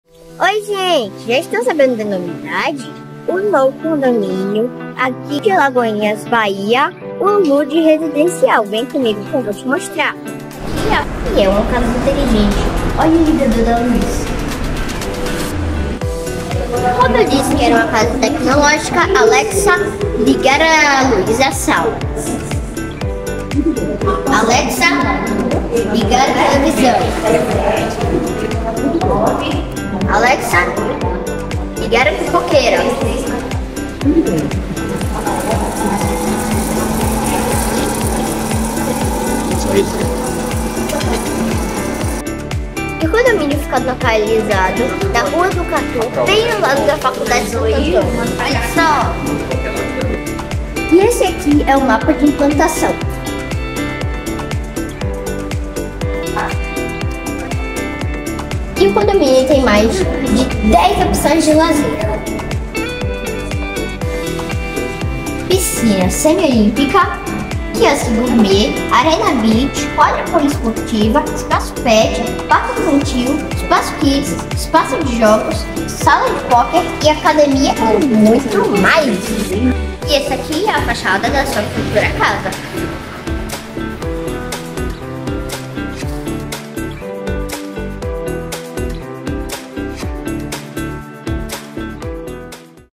Condomínio Residencial Luggi Alagoinjas, o Youtuber @Naomeignore.727Apresenta para voce.
O Youtuber Rafael Oliveira apresenta o Lançamento Residencial Luggi Alagoinhas da VCA Construtora | Assista e conheça os detalhes desse empreendimento, mais informações (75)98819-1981